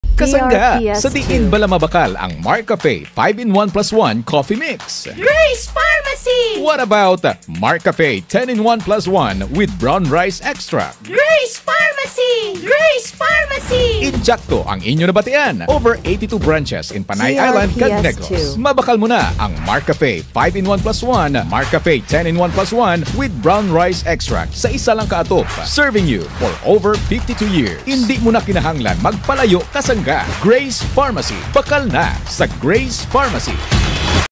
JMAR Herbs Products Radio Ad 2019-2024 34s (Grace Pharmacy Availability Version)
Mabibili na ang mga produkto ng JMAR Herbs sa lahat ng sangay ng Grace Pharmacy.

DRPS Two 8068: JMAR Herbs Products Radio Ad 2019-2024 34s (Grace Pharmacy Availability Version)